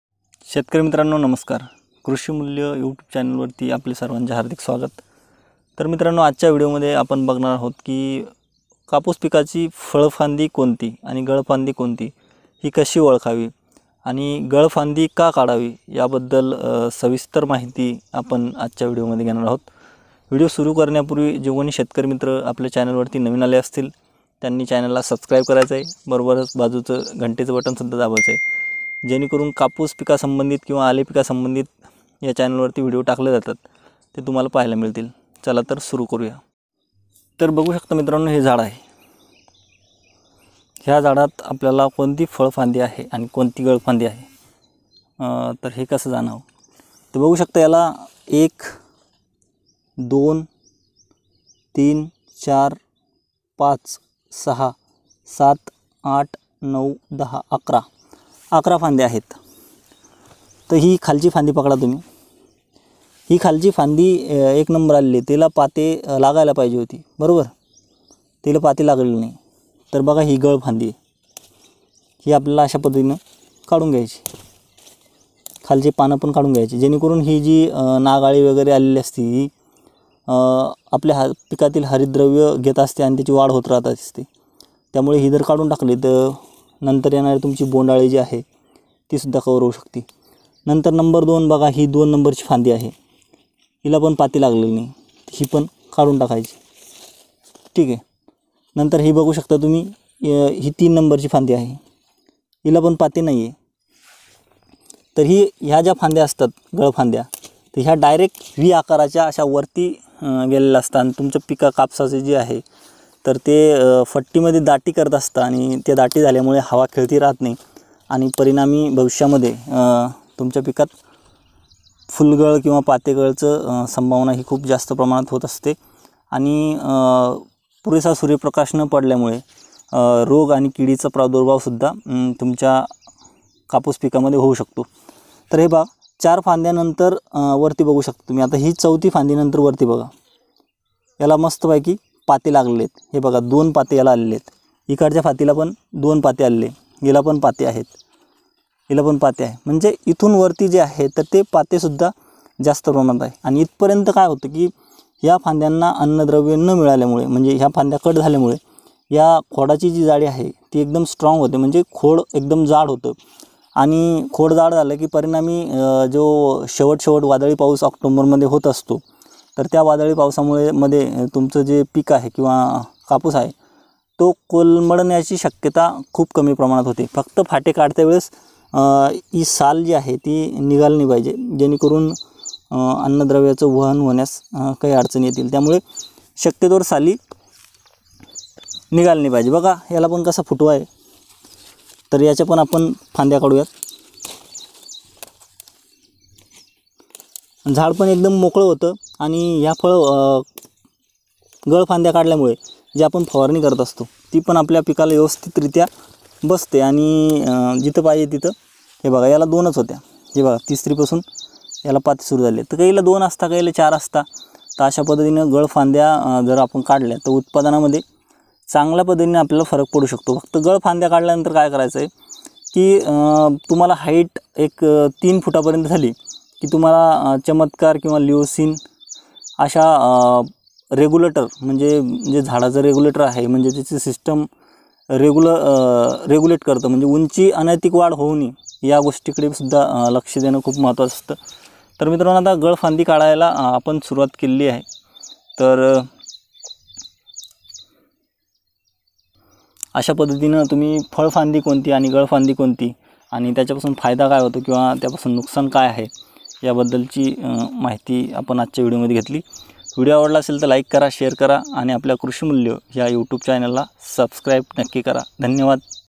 कपाशी पिकातील फळ फांदी कोणती व गळ फांदी कोणती, kapus pikatil fal fandi konti v gal fandi konti
Namskar  शेतकरी मित्रांनो कृषीमुल्य चॅनल वर आपले स्वागत आहे़ आपणास या विडिओ मध्य
आपण बघणार आहोत की कपाशी पिकातील झाडाची फळ फांदी कोणती व गळफांदी कोणती फळ फांदी काढणीमुळे कापूस उत्पादनामध्ये काय फरक पडू शकतो त्याचबरोबर त्यापासून काही नुकसान होते का त्या संपूर्ण विषयावरती सविस्तर चर्चा या व्हिडिओमध्ये करण्यात आले आहे त्याचबरोबर फळ फांदी गळ फांदी काढण्याची योग्य वेळ कोणती आहे त्याबद्दल सुद्धा सविस्तर माहिती देण्यात आली आहे तुम्ही जर कापूस उत्पादक शेतकरी असाल तर हा व्हिडिओ शेवटपर्यंत नक्की बघा

कृषीमुल्य
शेती विषयक माहिती